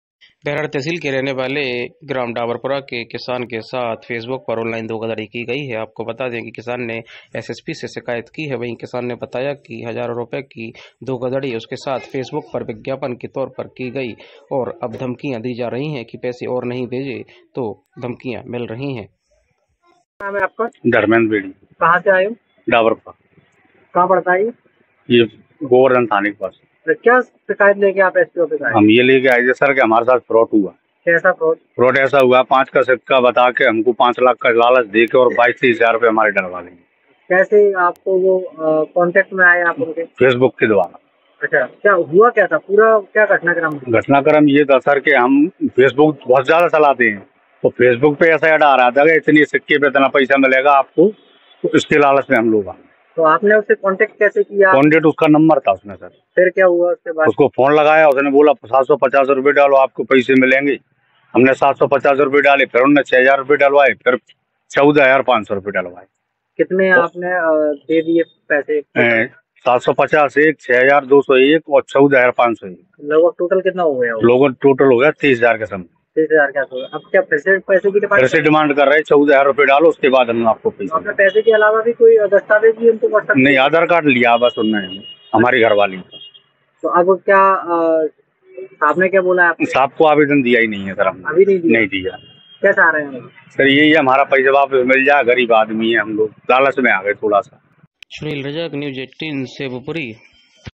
Online Fraud Case: फेसबुक पर एड देखकर फंसा किसान और लग गई भारी चपत
शिवपुरी के बैराड़ तहसील के अंतर्गत आने वाले ग्राम डाबरपुरा के रहने वाले किसान ने बताया कि उसके साथ ऑनलाइन धोखाधड़ी की गई है उसने फेसबुक पर ऐड देखा था और उसी के एवज में उसके साथ धोखाधड़ी की गई किसान ने एसएसपी से की है.